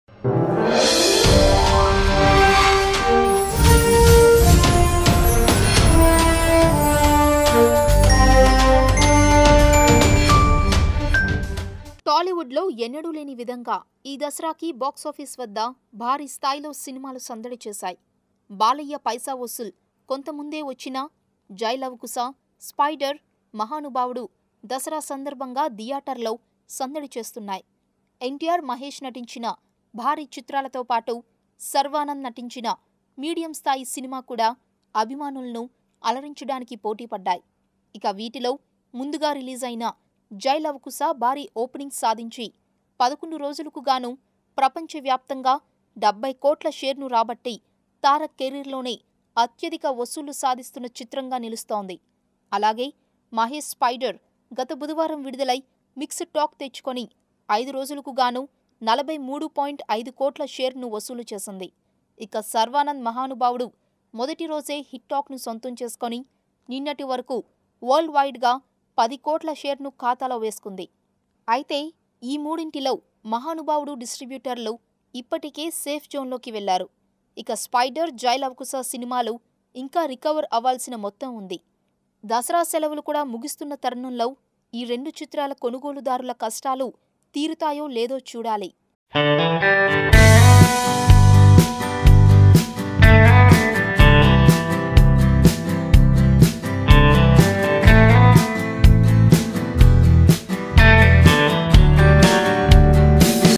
3 Movies Box Office Collections || Dussehra Hit Movies || Spyder || Jai Lava Kusa || Mahanubhavudu
3 Movies Box Office Collections. Sharwanand film turns Dussehra Hit,Mahanubhavudu 3 Day Worldwide Box Office Collection. Spyder ,Jai Lava Kusa , Have to reached their collections. watch latest videos and more updates.

ENTERTAINMENT

MOVIES

NEWS

WEBSITES